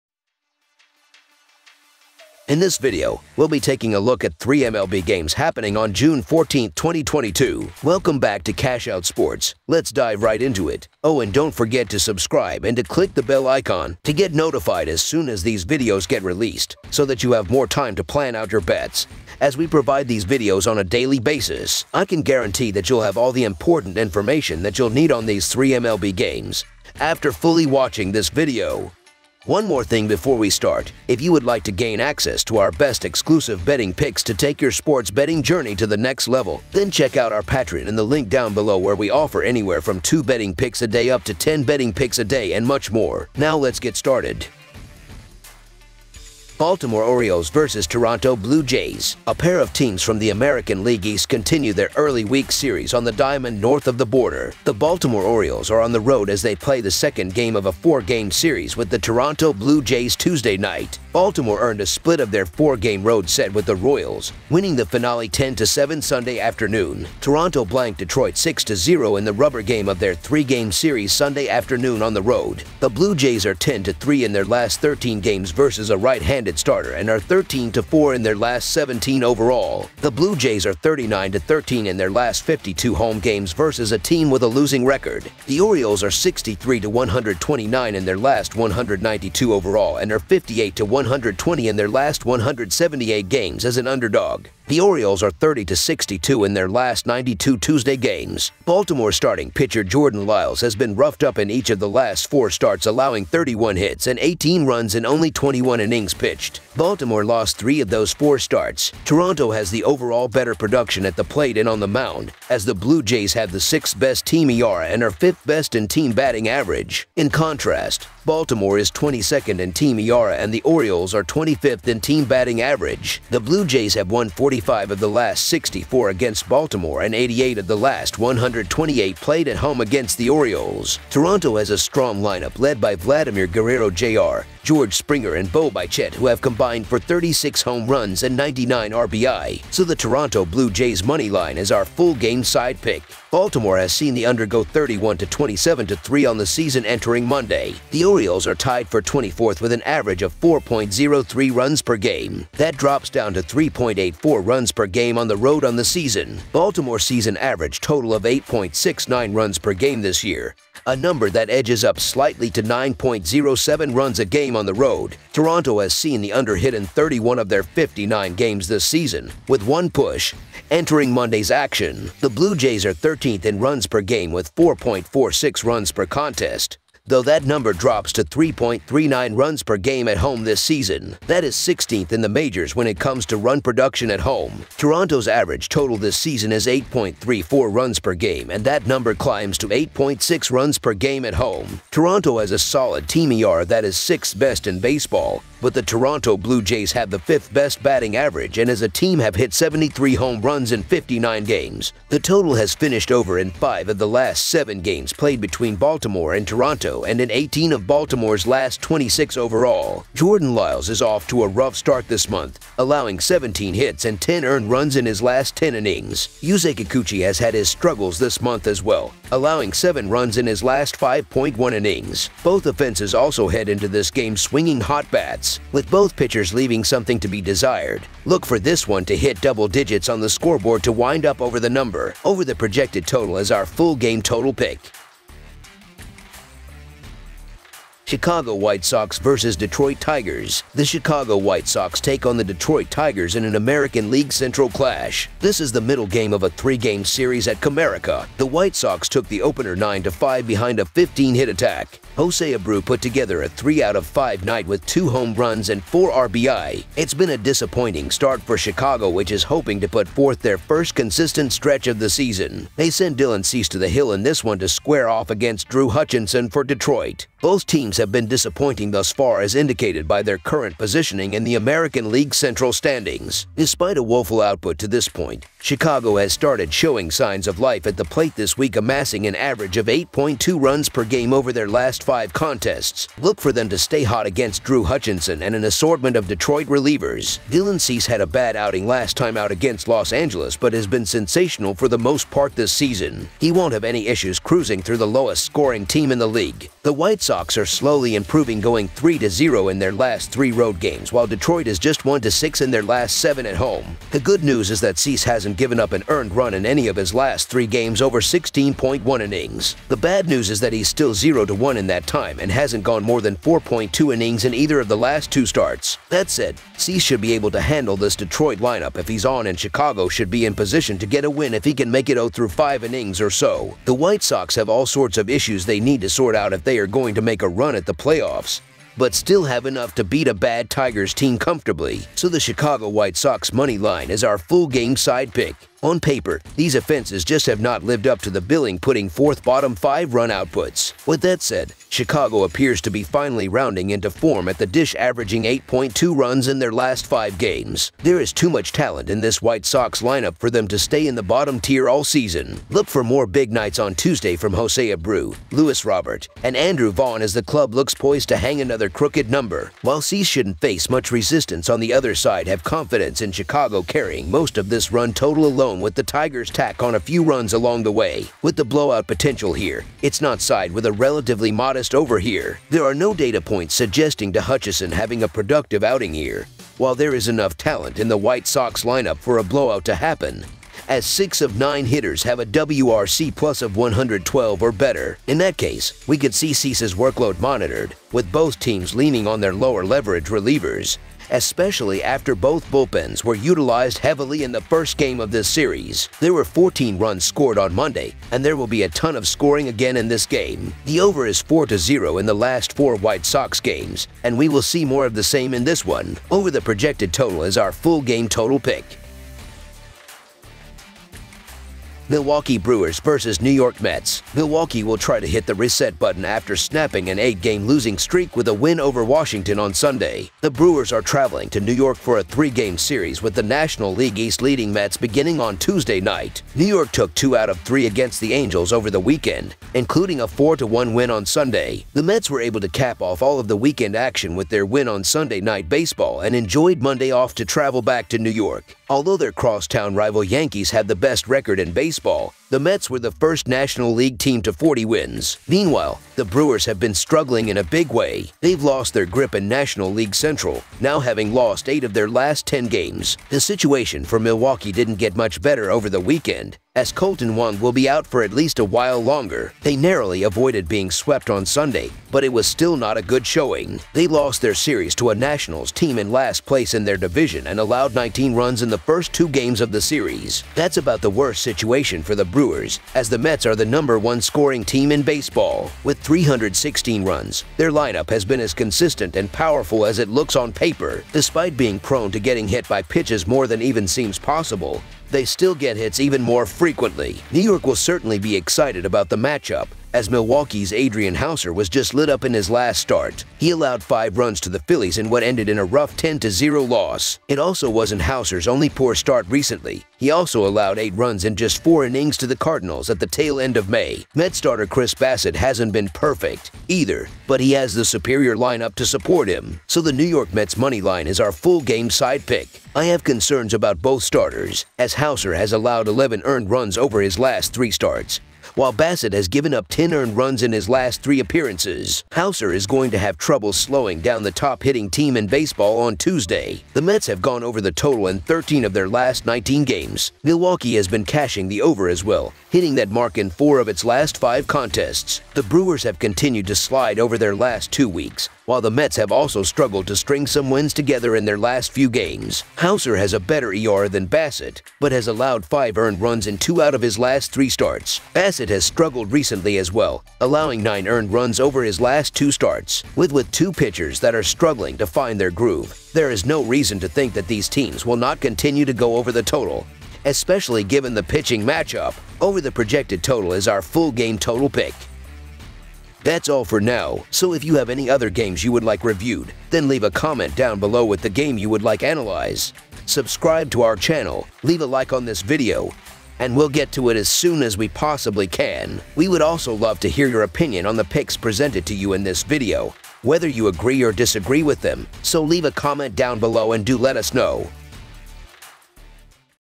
FREE MLB Picks and Predictions Today 6/14/22 Baseball MLB Betting Tips and Analysis
Baltimore Orioles vs. Toronto Blue Jays
Chicago White Sox vs. Detroit Tigers
Milwaukee Brewers vs. New York Mets